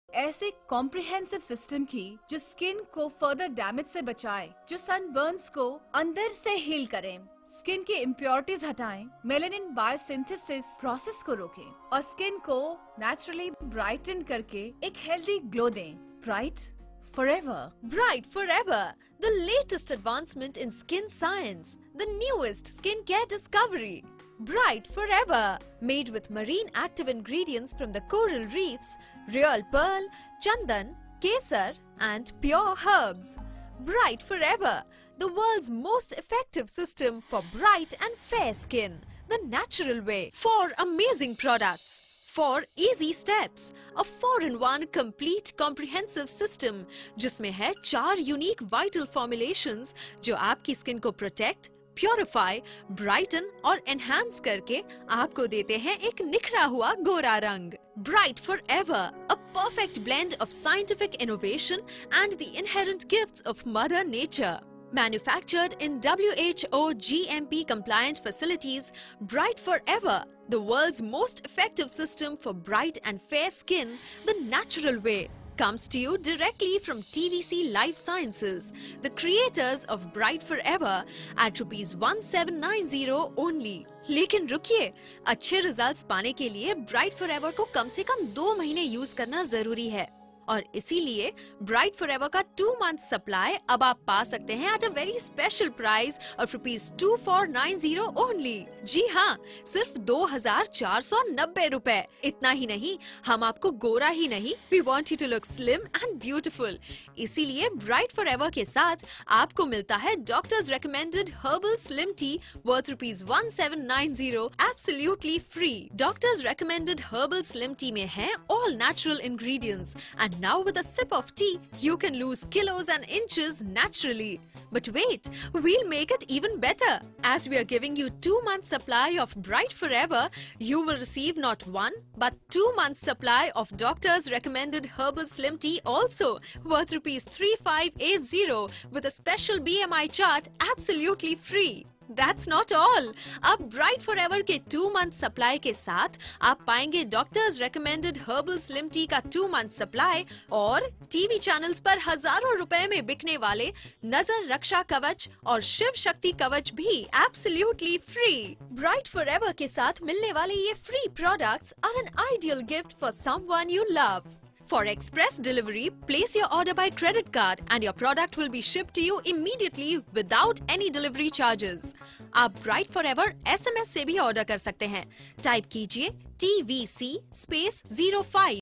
TVC Bright Forever - For best fairness cream and herbal treatment for skin
Bright Forever is the world's most effective system for bright and fair skin, the natural way. It uses the latest advancements in skin science & the newest skin care discovery, made from marine actives.

Bright Forever is enriched with real pearl and is a perfect blend of scientific innovation coupled with the inherent gifts of Mother Nature.